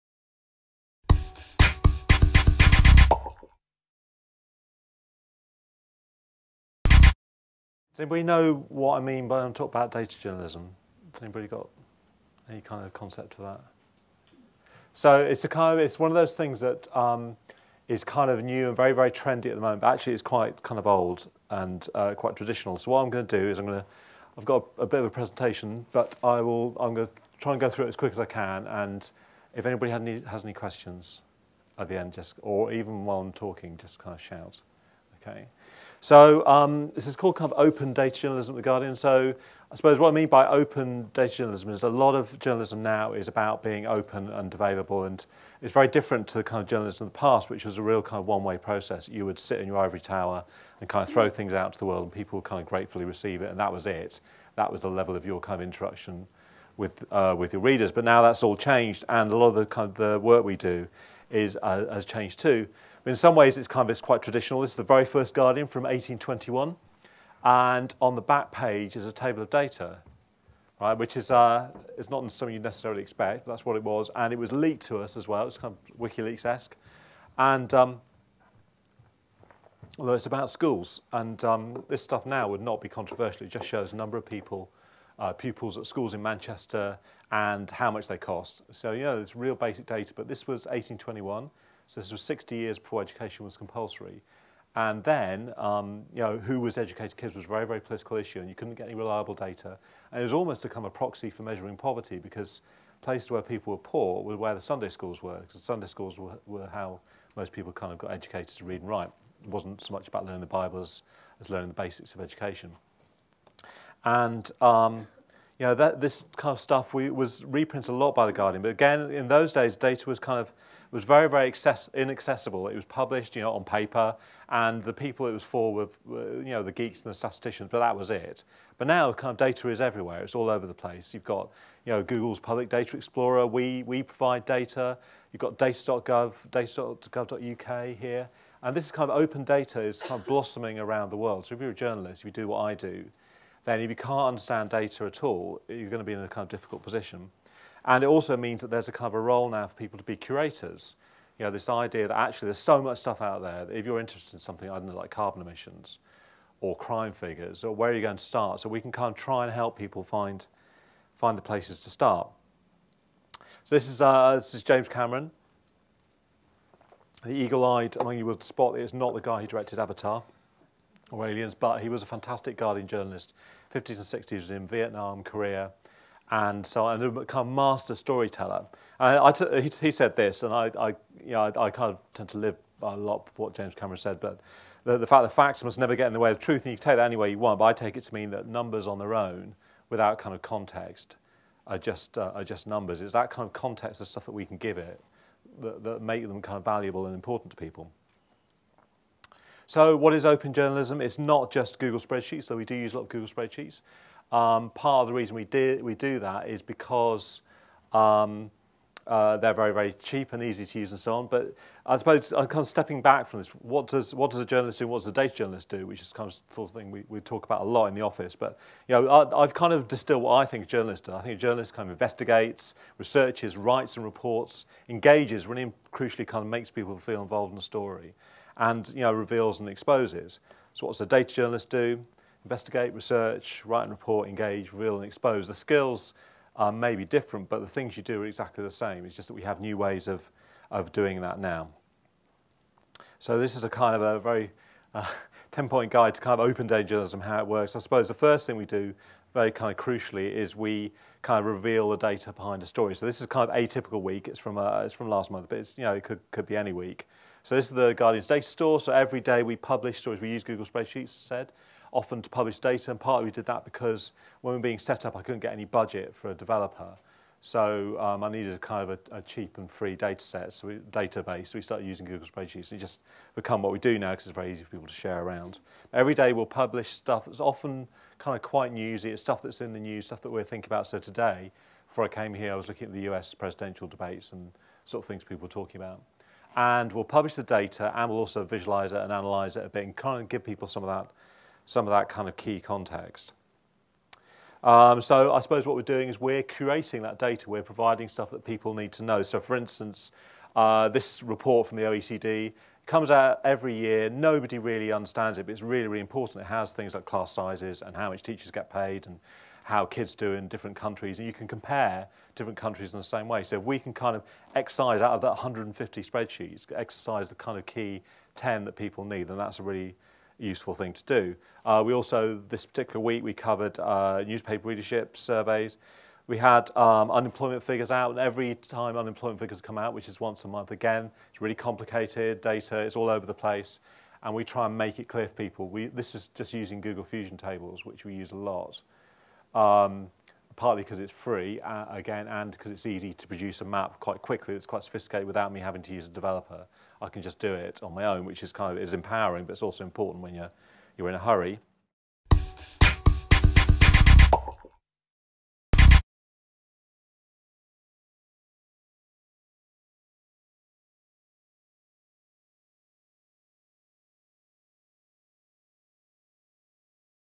Simon Rogers Squared Online Course Video Excerpt
Named Best UK Internet Journalist by the Oxford Internet Institute and having been launch editor of the Guardian's online news service, Simon Rogers now leads Guardian.co.uk/Data -- probably the world's most popular data journalism website. In this session he discusses how digital has changed the way media is created and consumed.

This is an excerpt from a video lecture unique and exclusive to Squared Online - an online curriculum in digital marketing developed with Google and powered by Home Learning College.